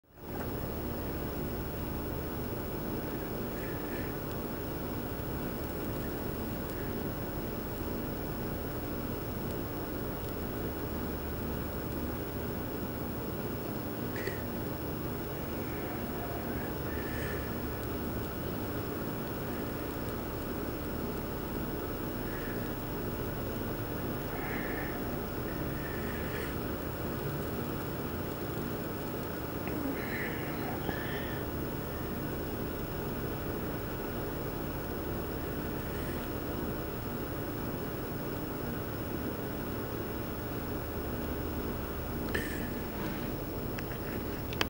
Fire Block vs. Blowtorch
A bit of fire block being burned with a blowtorch.  This is sort of a foamy-rubbery material, with properties otherwise similar to the fire caulk and fire pad materials.  It reacts pretty much the same to fire as the other materials, but it seems to more strongly block the flame from going very deep into it.